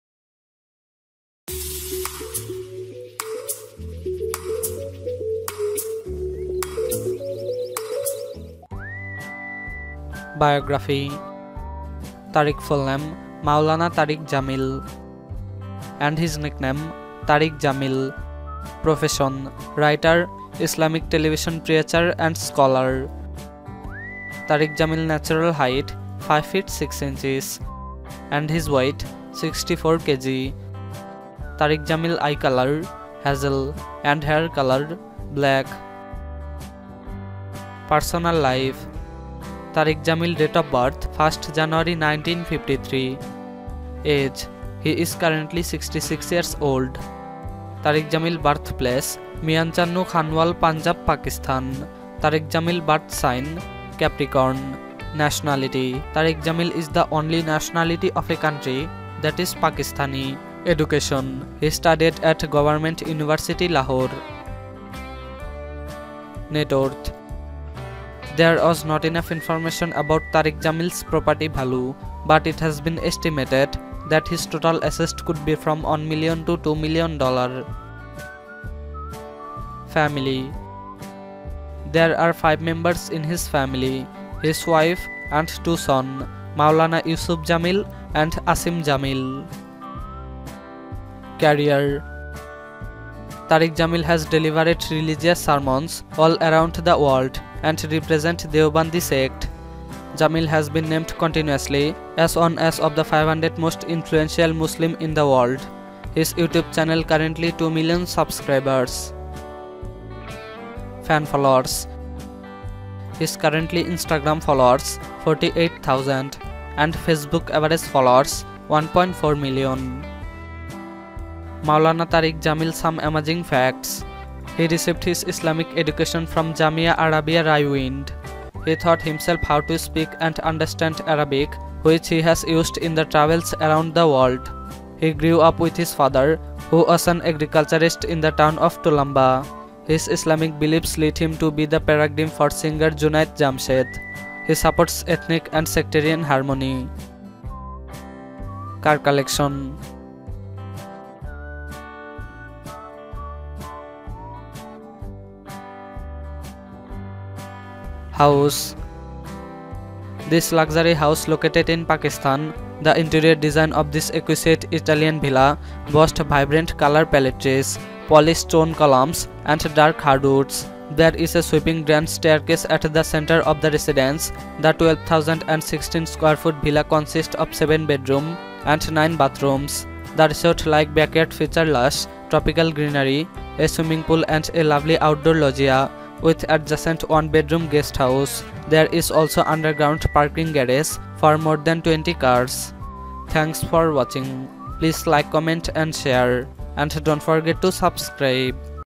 Maulana Tariq Jamil (طارق جمیل‎;)Biography,Net Worth,Income,Family,Cars,House & LifeStyle
Tariq Jamil  born 1 October 1953 is a Pakistani Islamic television preacher, religious writer, scholar and a member of the Tablighi Jamaat
Jamil is an alumnus of Government College University, Lahore. He received his Islamic education from Jamia Arabia, Raiwind, where he studied Qur’an, Hadith, Sufism, logic, and Islamic jurisprudence